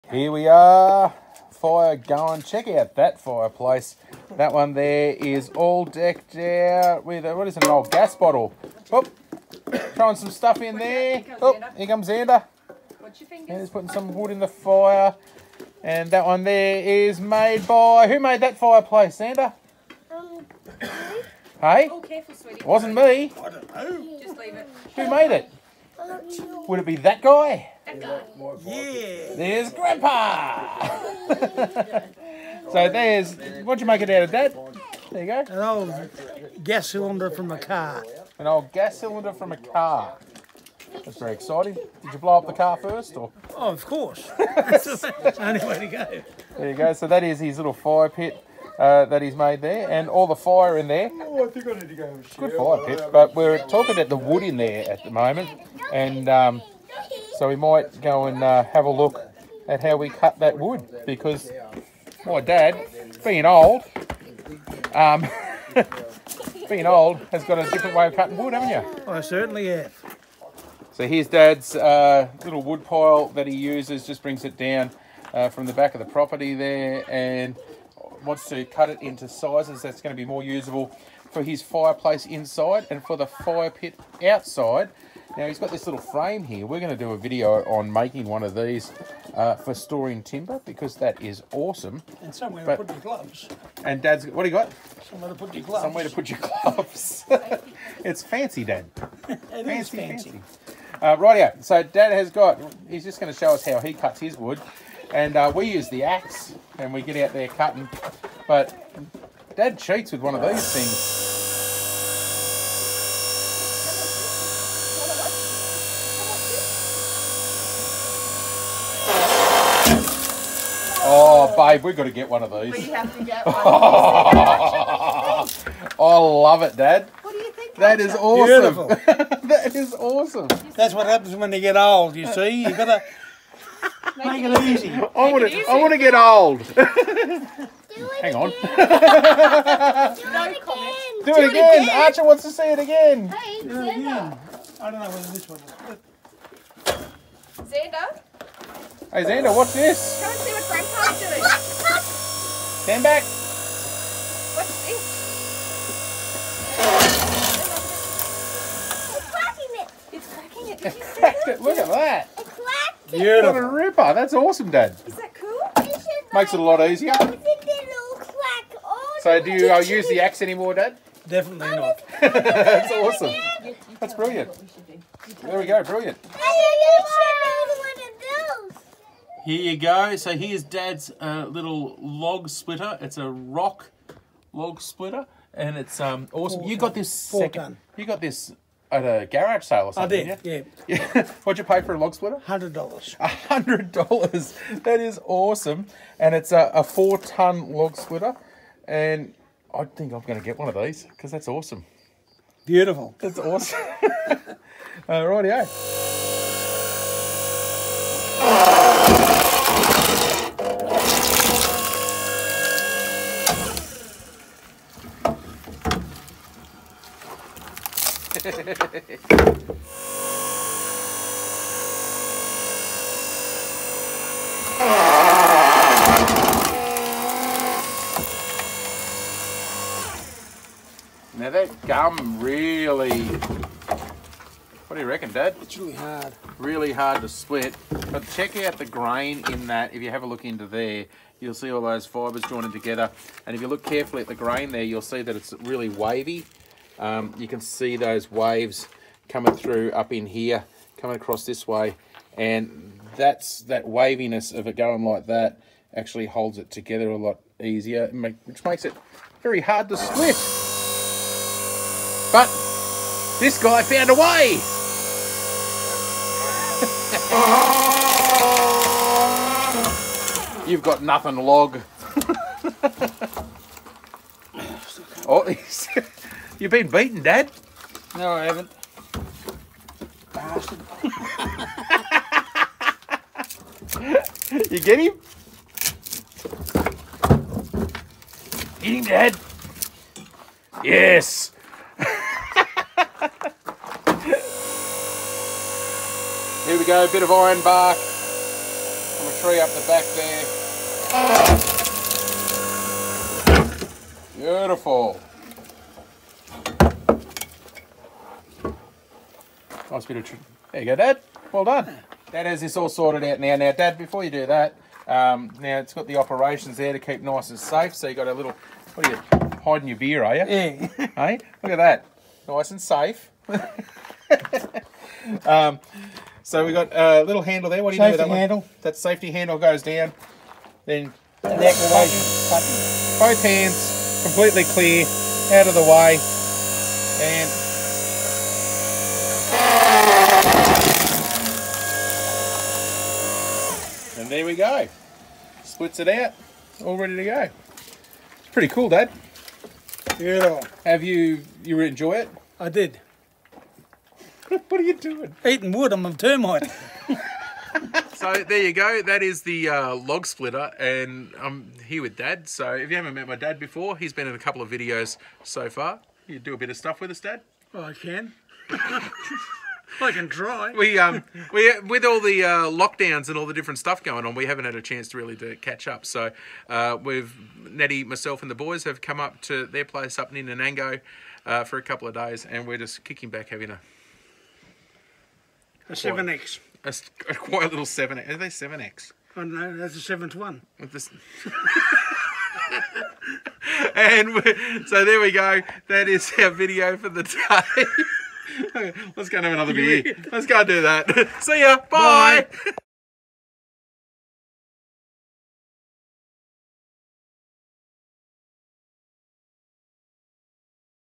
ROK LOG SPLITTER
Visiting Mum and Dad we got a chance to check out Dads log splitter that he picked up at a garage sale.